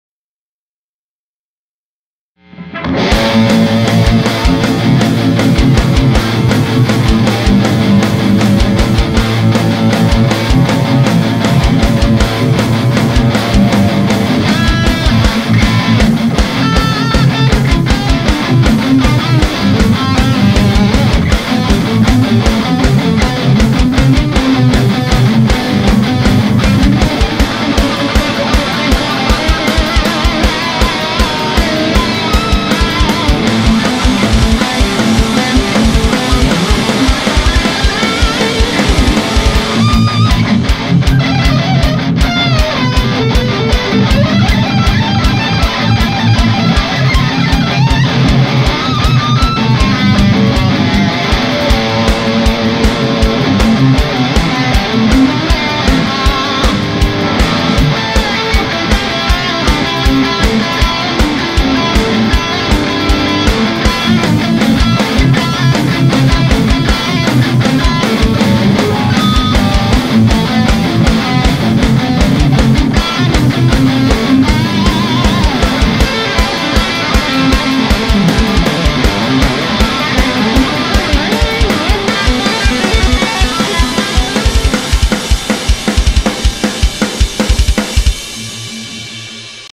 Impromtu MONO jam over new riff. JCM800 and TS9
Off the cuff riff and solo.  Mixed in mono by mistake so it's a bit flat.  But everything sounds good with this combo.  Even random flailing.  No bass.  Or mix...Or sanity.